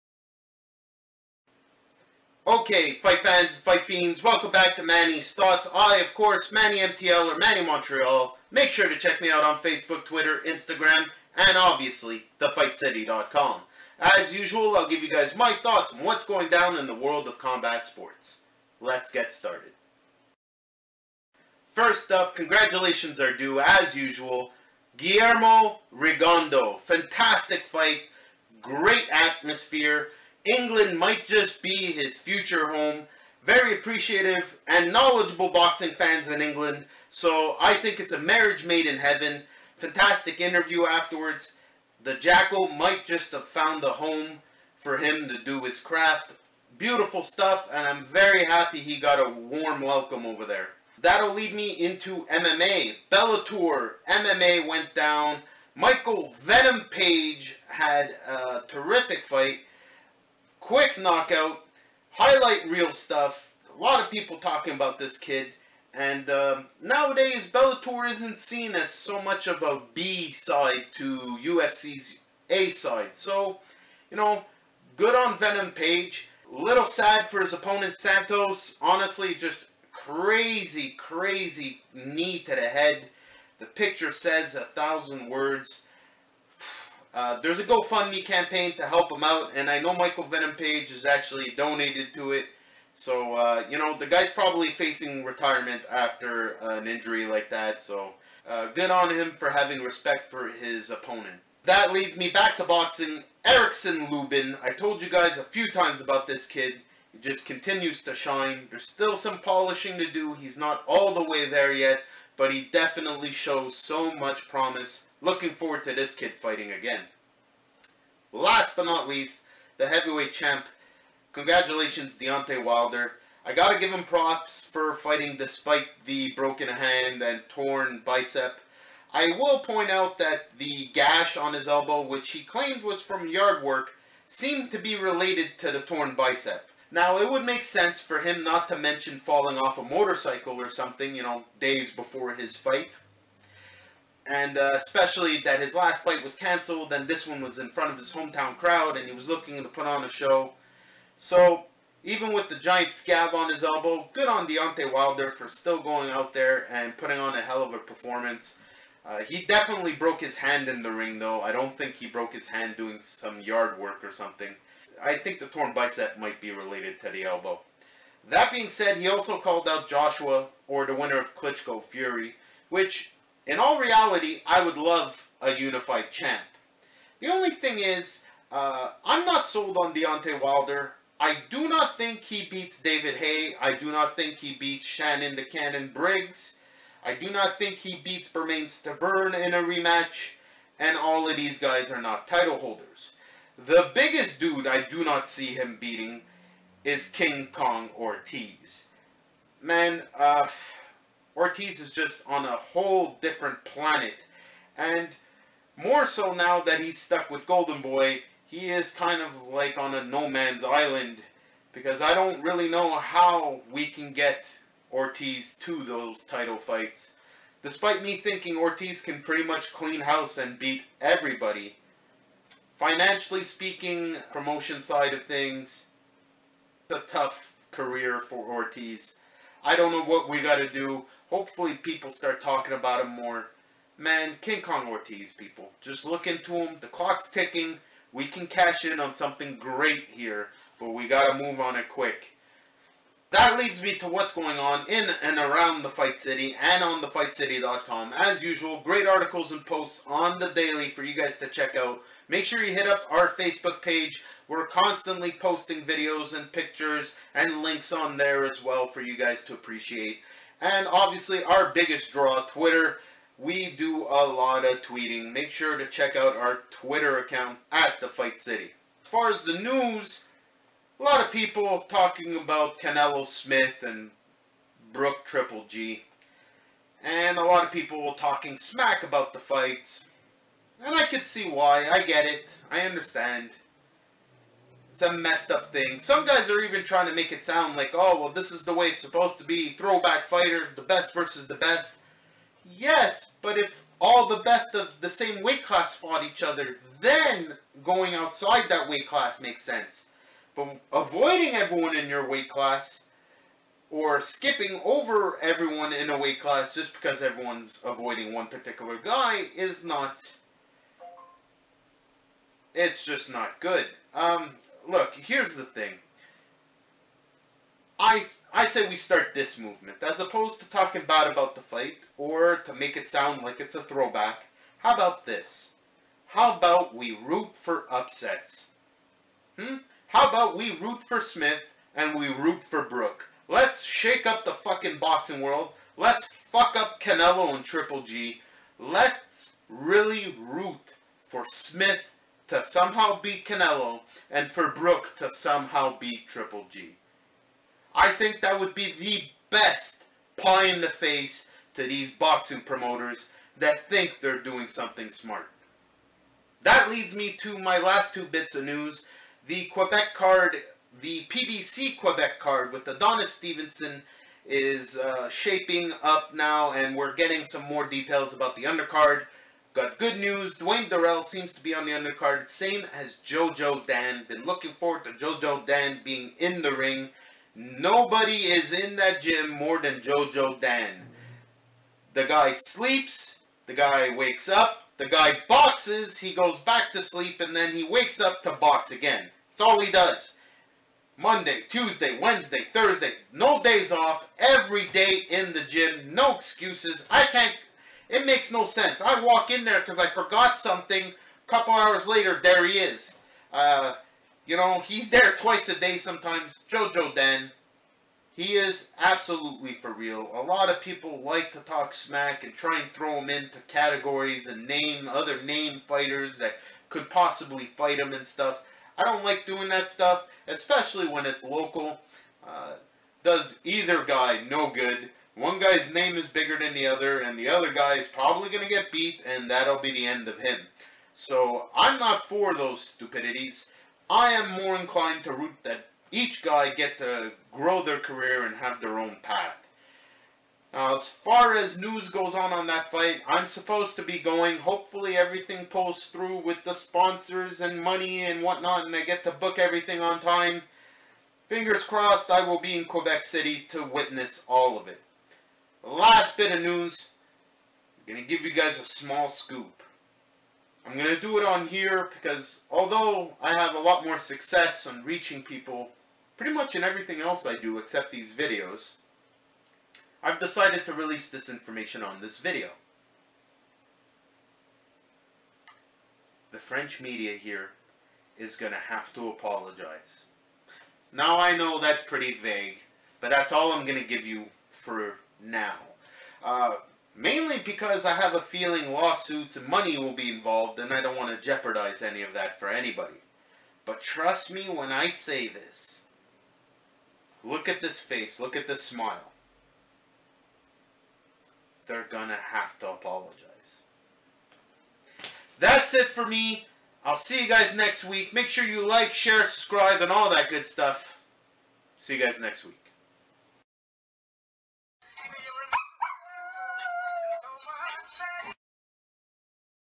Manny's Thoughts Boxing Mma Montreal
Time again for Manny’s Thoughts! From Manny Montreal! This week he’s talkin’ Guillermo Rigondeaux getting a warm welcome on the other side of the ocean! The UK’s Michael ‘Venom’ Page! Good luck to Santos! Eric Lubin looking good! Deontay Wilder and his injuries! Hoping for big fights for King Kong Ortiz! Manny rooting for Smith and Brook! Jo Jo Dan and Dwayne Durel fighting on the PBC Quebec City undercard! Cryptic sign-off from Manny at the end! Check it out:

Twitter: MannyMtl Facebook: Manny Montreal